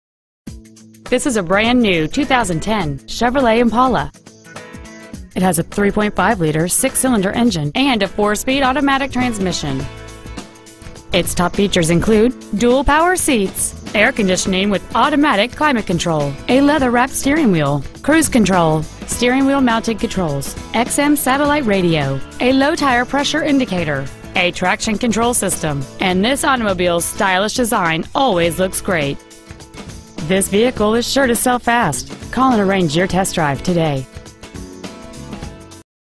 2010 Chevrolet Impala Toccoa GA
Year: 2010
 Make: Chevrolet
 Model: Impala
 Engine: Gas/Ethanol V6 3.5L/214
 Trans.: Automatic
 Exterior: Silver Ice Metallic
 Miles: 11
 Interior: Grey

 http://

 1223 S. Big A Road

 2010 Chevrolet Impala Toccoa GA